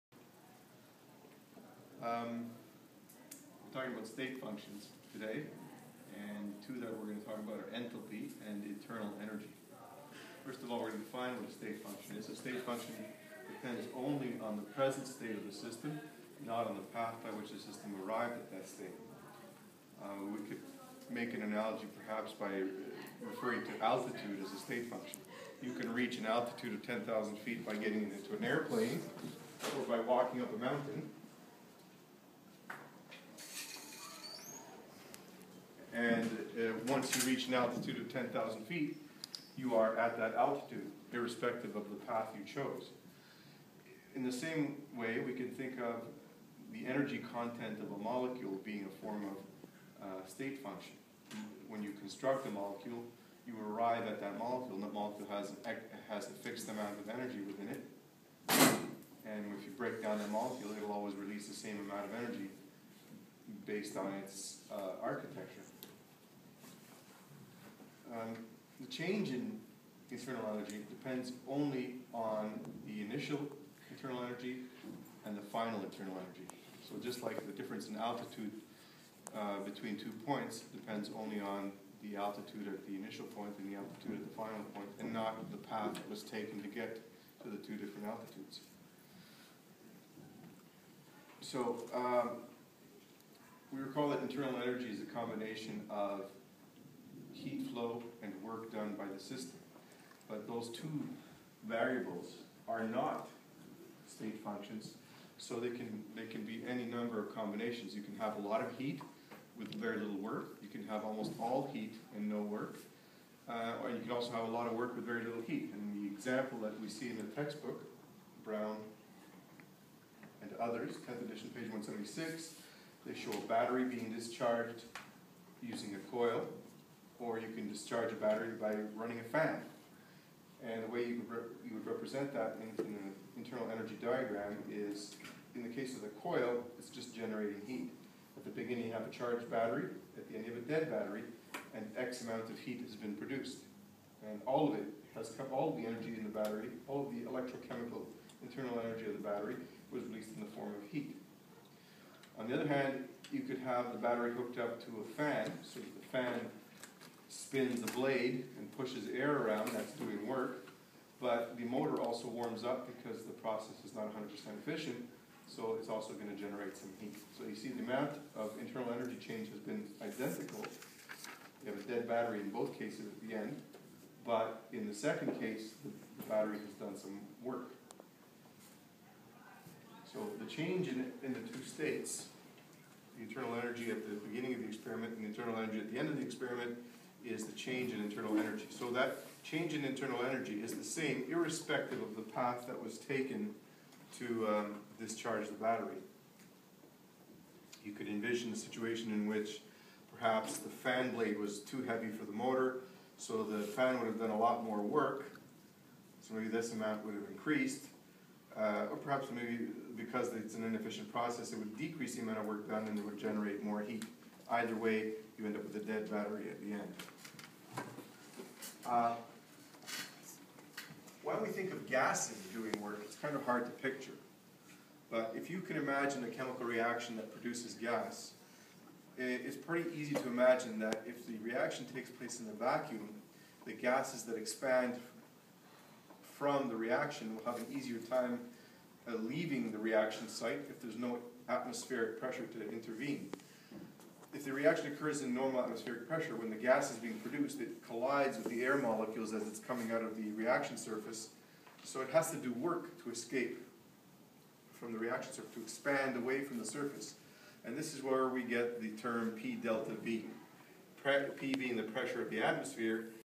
Thermodynamics P.3: Internal Energy Manifest as Heat and Work
Internal energy is a state function. The internal energy of a given molecule is always the same, and it is based on the architecture of the molecule. The internal energy change between two substances is also the same, irrespective of the path chosen. Internal energy represents a chemical reaction's or substance's energy content. Changes of internal energy usually manifest as an output (or input) of both heat and mechanical work, but the sum of those two quantities will add up to the difference of internal energy between two discrete states. As a practical example: Two different engines burning a given quantity of fuel rated as capable of producing 100 Joules of energy can still have different work outputs depending on their efficiency. The more efficient engine will convert more of the fuel's energy into mechanical work and will thereby produce less waste heat. Thus the engine producing 20J of work will produce 80J of waste heat while the engine that generates 12J of work will produce 88J of waste heat from the same amount of fuel. An explanation of work produced by expanding gases follows.